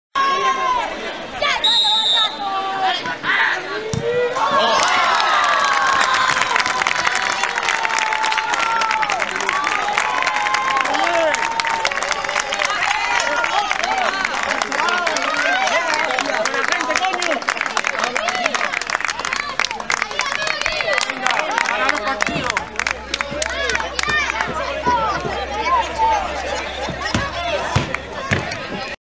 Gol de Ángel Rodríguez, partido contra el Atlético de Madrid B Temporada 2007-2008
Gol de Ángel Rodríguez, 1-0 (de penalti), minuto 41. Partido de fútbol UD Villa Santa Brígida-Atlético de Madrid B. Segunda División B. Grupo I. Jornada 14. Temporada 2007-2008. Campo de Fútbol del Guiniguada. Santa Brígida. Resultado final: 1-1.